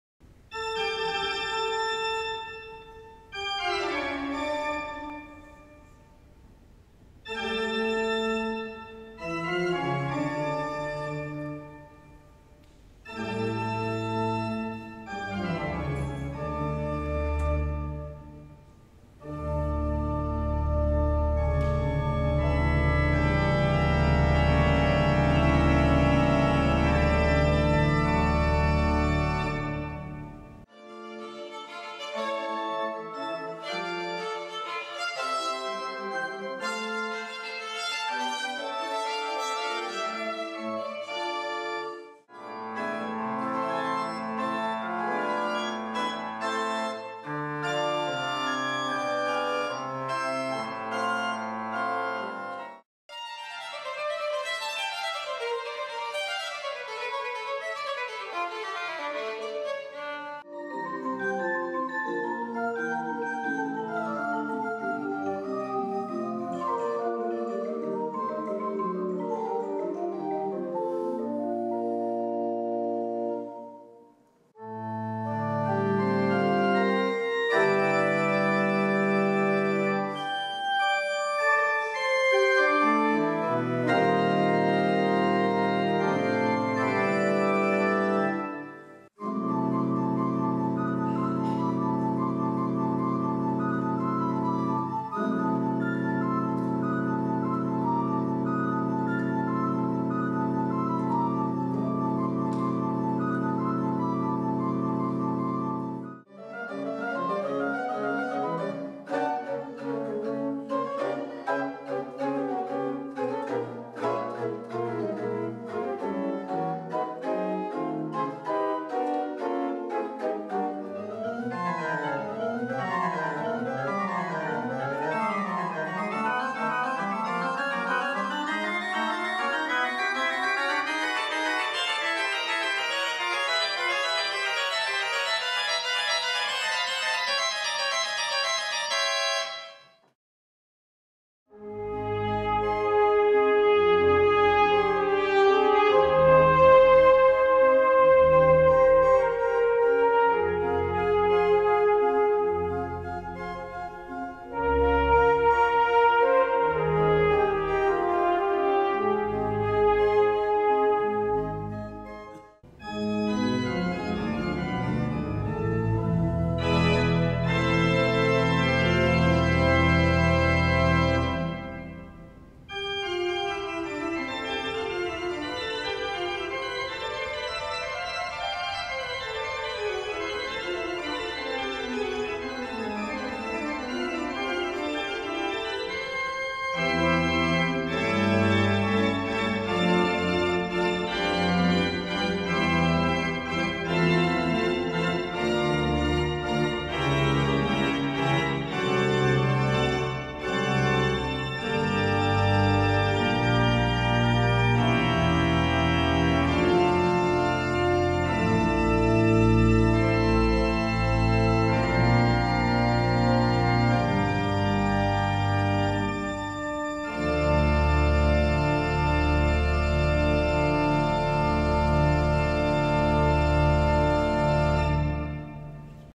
Orgelherbst 2017
Ein kurzes Video vom "Orgelherbst 2017" in Bruck/Leitha-Hainburg/Donau